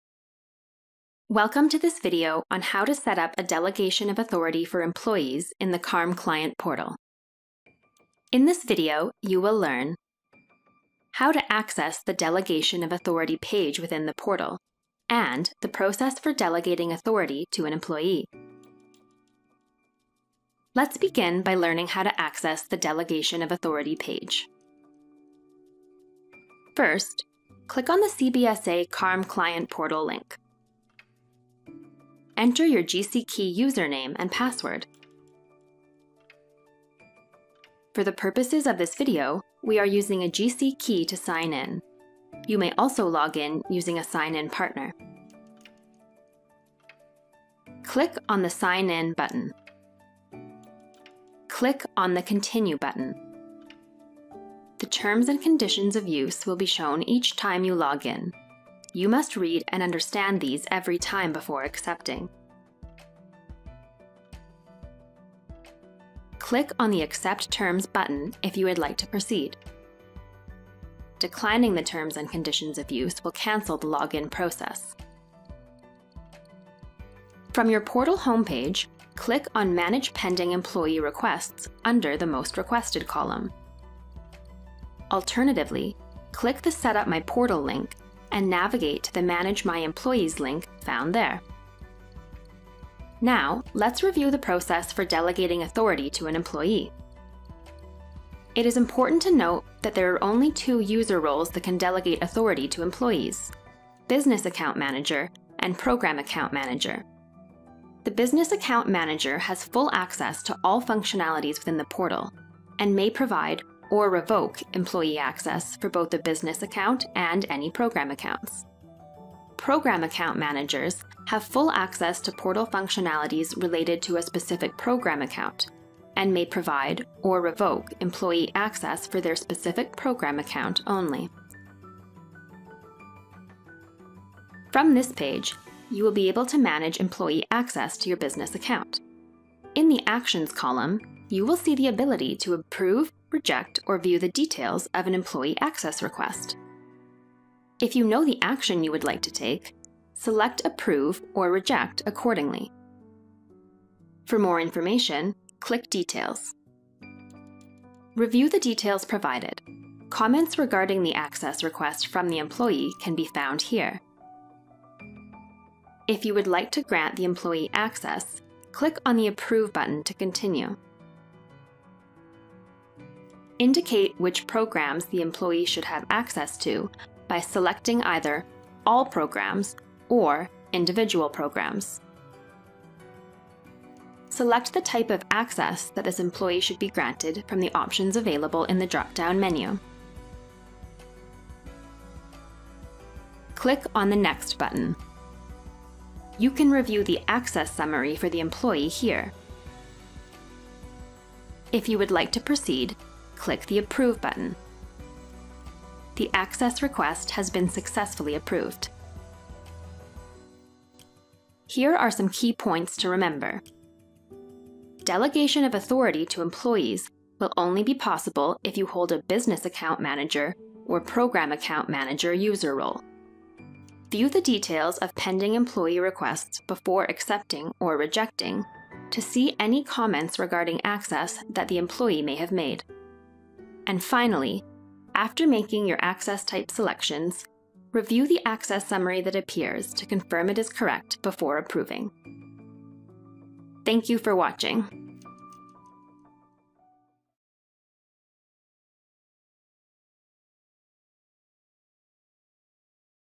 OLDHow to set up a delegation of authority for employees in the CARM Client Portal
In this video you will learn how to access the delegation of authority page within the portal and the process for delegating authority to an employee.